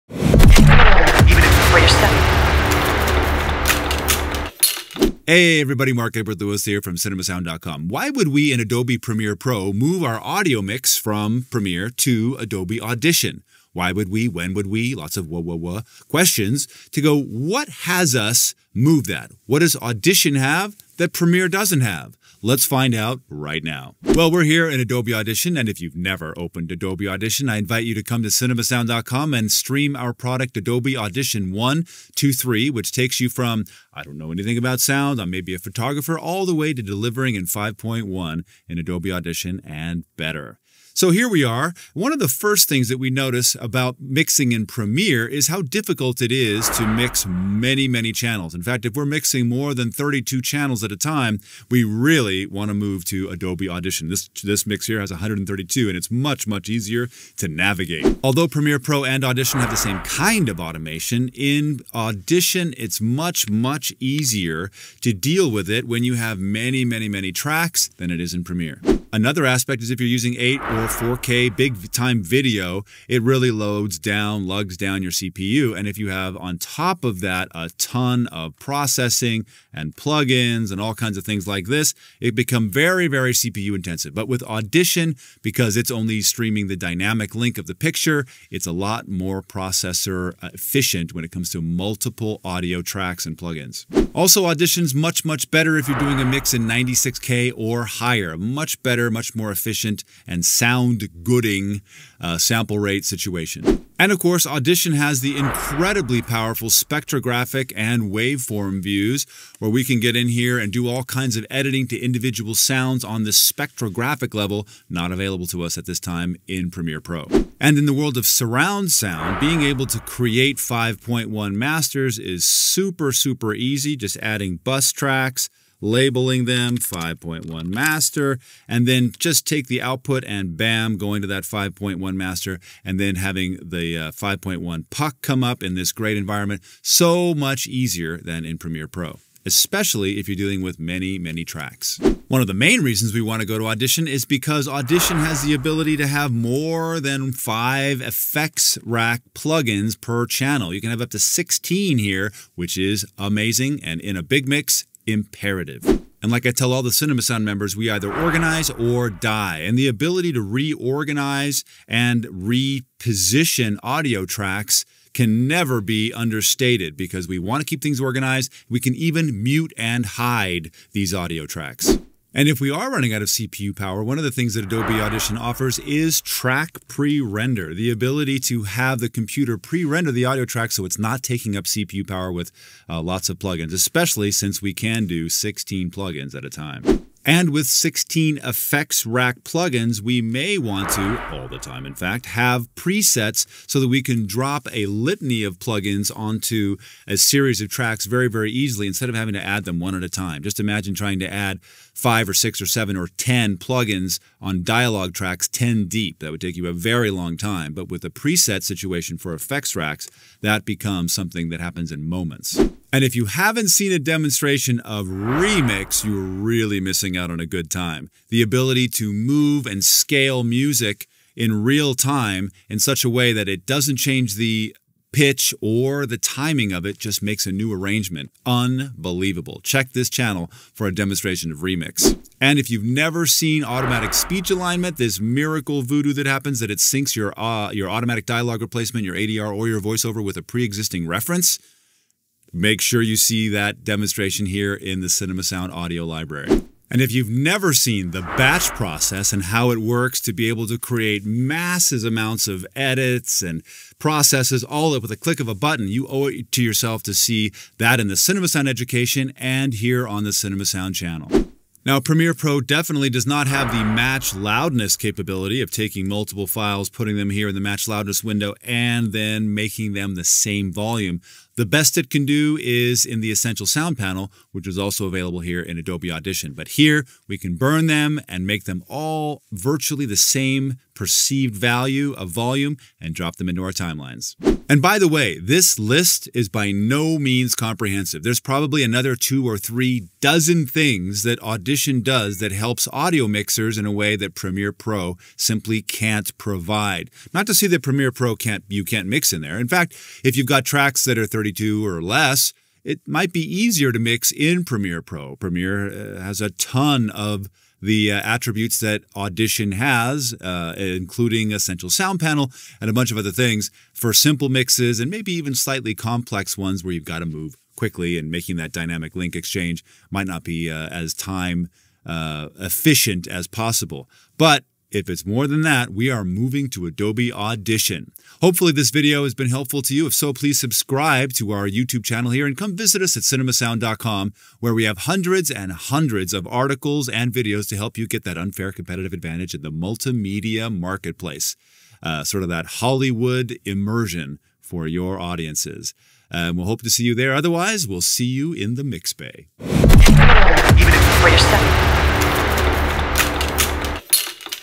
Why Do We Leave Adobe Premiere Pro?
In this video Cinema Sound main presenter Mark Edward Lewis shows us HOW and WHY we would leave Adobe Premiere Pro and go to Audition for audio mixing and editing.

Discover how you can bring Hollywood-Level audio immersion into your productions by going